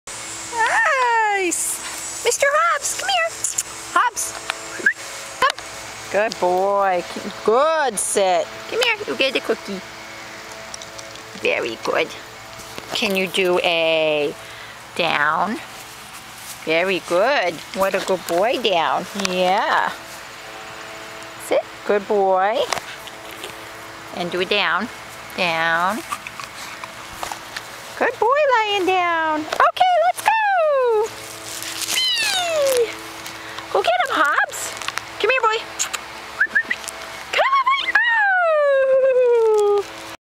Hobbs is a mini aussie shepherd. He's up for adoption through Paws New England.
Hobbs is up for adoption through Paws New England-